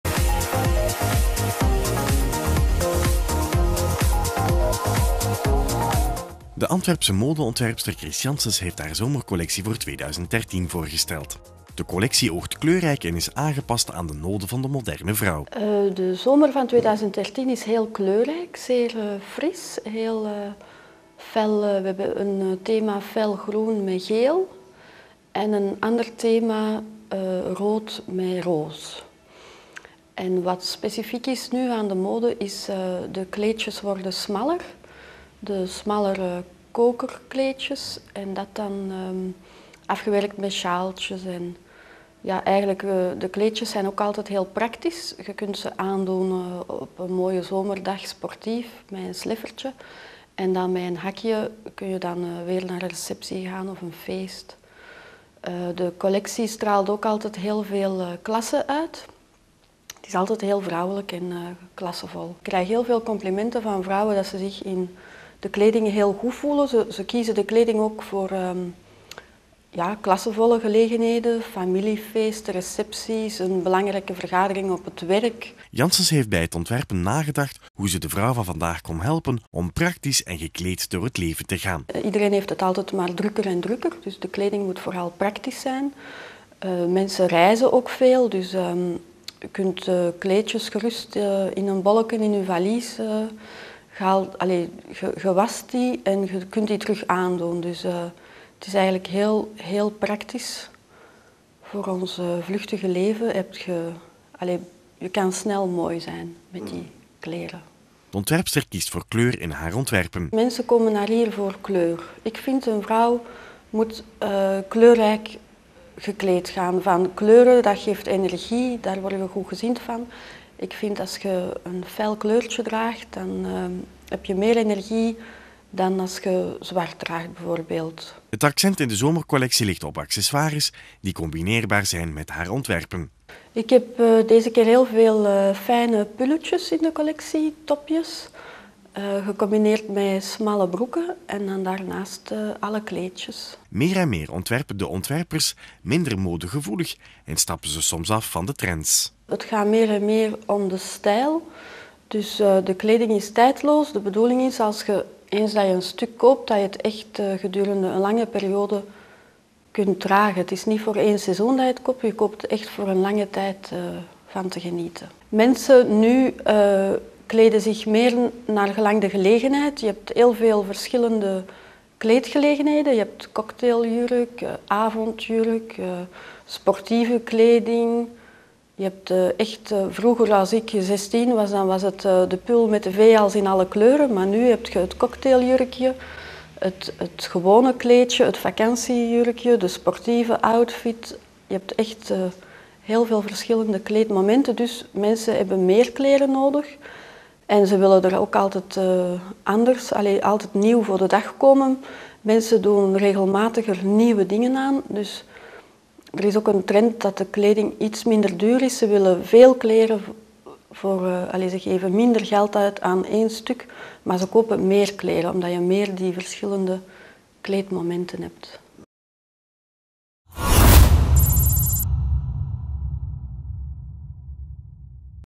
Chris Janssens
De Antwerpse Modeontwerpster Chris Janssens heeft haar zomercollectie voor 2013 voorgesteld.
De collectie oogt kleurrijk en is aangepast aan de noden van de moderne vrouw.
Janssens heeft bij het ontwerpen nagedacht hoe ze de vrouwen van vandaag kon helpen om praktisch en gekleed door het leven kunnen gaan.
De ontwerpster kiest voor kleur in haar ontwerpen.
Het accent in de  zomercollectie ligt op accessoires die combineerbaar zijn met haar ontwerpen.
Meer en meer ontwerpen de ontwerpers minden modegevoelig en stappen soms af van de trends.

Beelden: De Kosmonauten